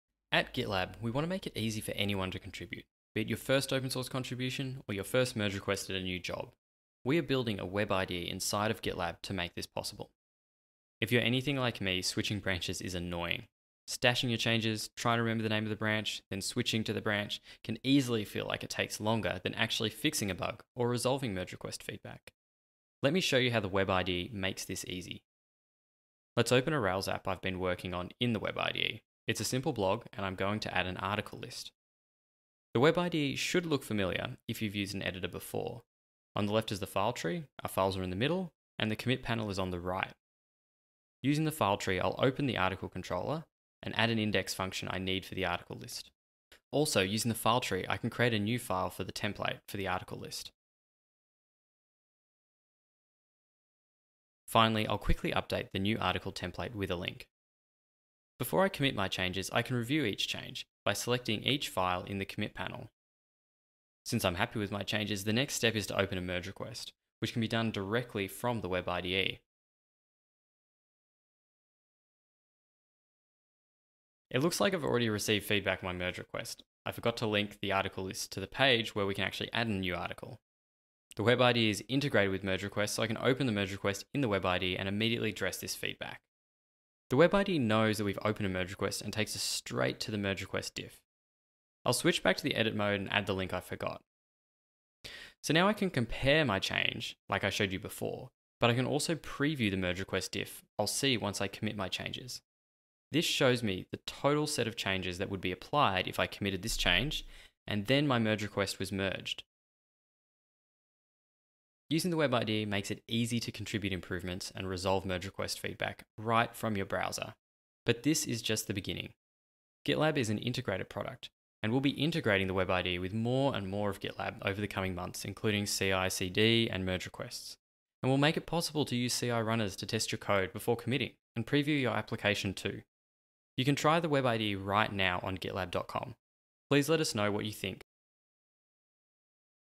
GitLab Web IDE now open source
The new Web IDE makes it faster and easier to contribute small fixes and resolve merge request feedback by eliminating the need to stash changes and switch branches locally.

Using the Web IDE, you can change multiple files, preview Markdown, review the changes and commit directly from the browser. The Web IDE can be opened when browsing the repository, viewing a file, and directly from merge requests so that you can quickly resolve feedback or contribute a change while doing a code review. If you open a merge request in the Web IDE you can also preview the merge request diff before committing, which allows you to review the entire scope of the merge request directly in the Web IDE!

Introduced in GitLab Ultimate 10.4, the Web IDE is now Generally Available (GA). We decided to make Web IDE open source, because we believe building something so complex and personal as an IDE is only possible together. Plus, this lowers the bar for anyone to contribute to their favorite projects.